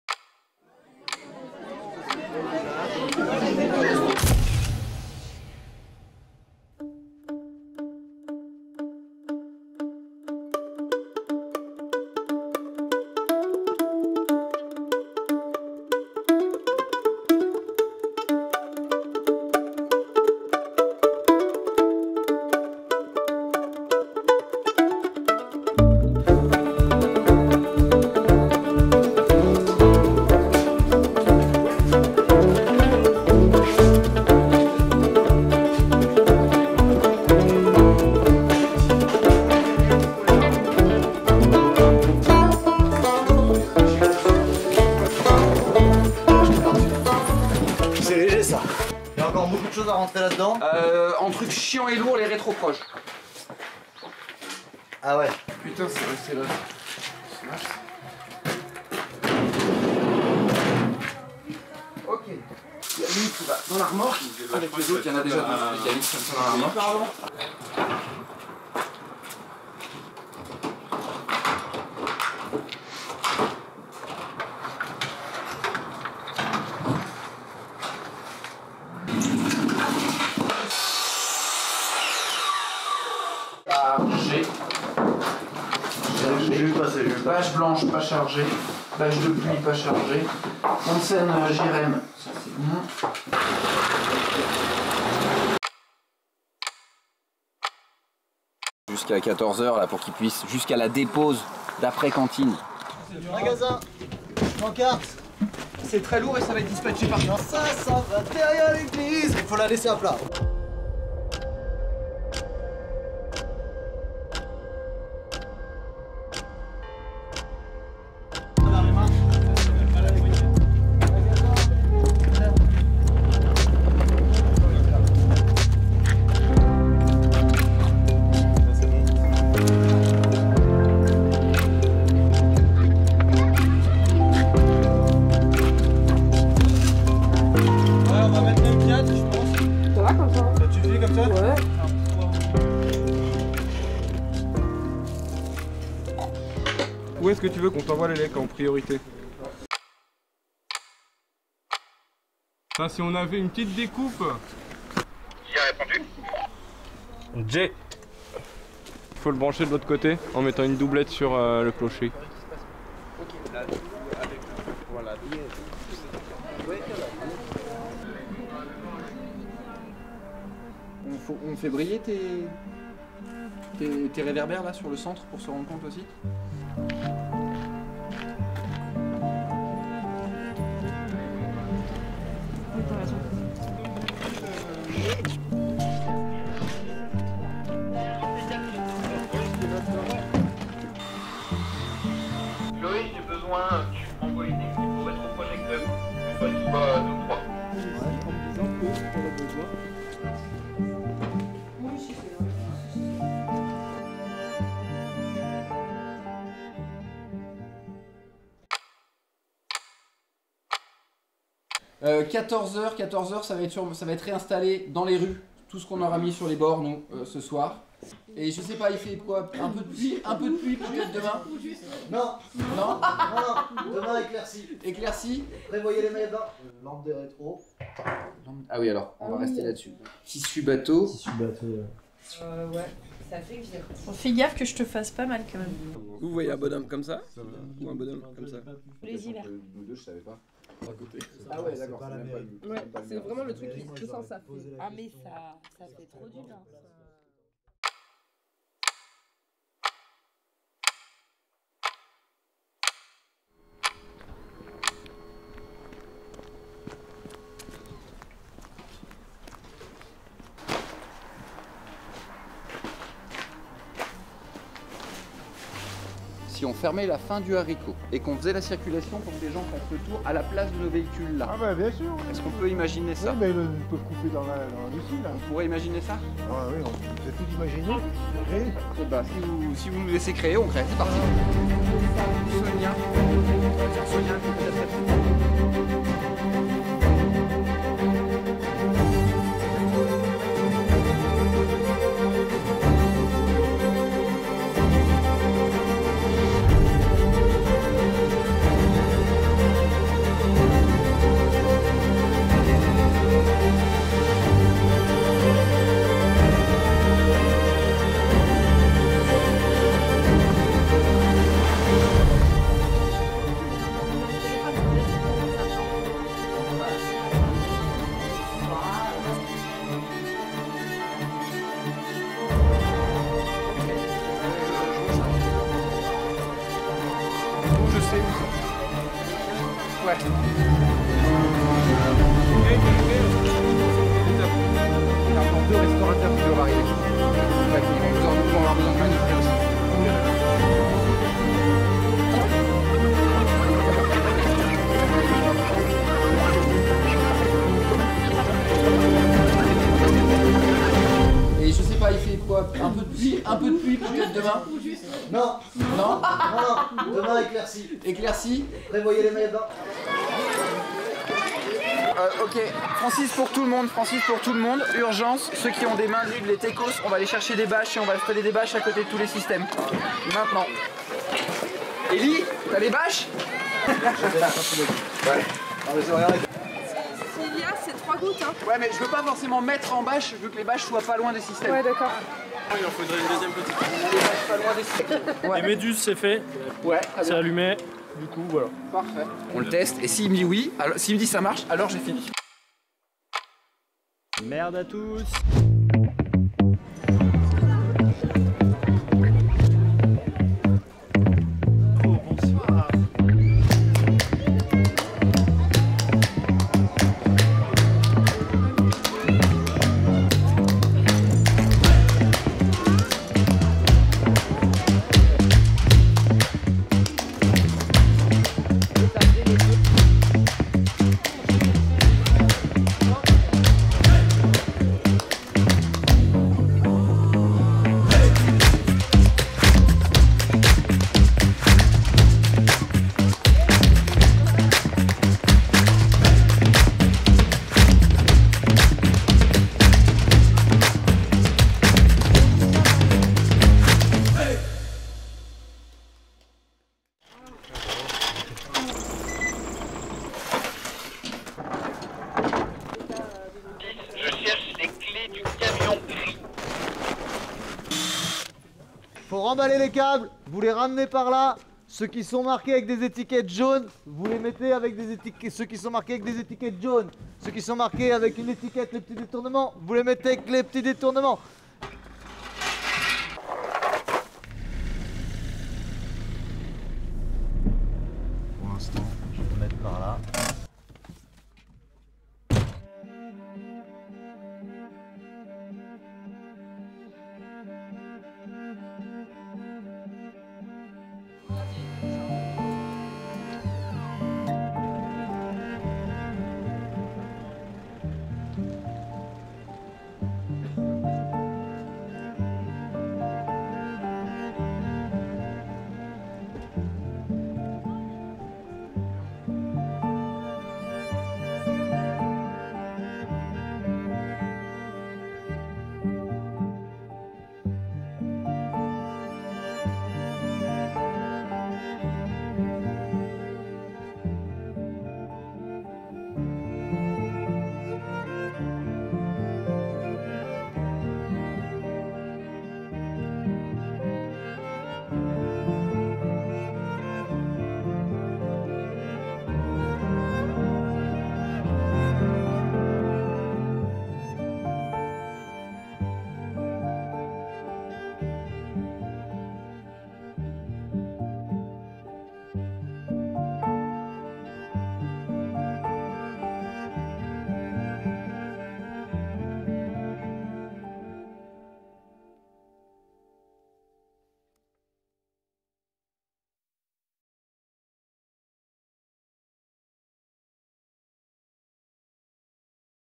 Soirée Bricoles et Lumignons - Saison 4
La compagnie les petits détournements est fier de vous présenter le documentaire réalisé par Ecurieux Film lors de notre 4ème édition de la fêtes des lumières du lac d'aiguebelette.

 2 décembre 2022 sur la commune de Saint Alban De Montbel.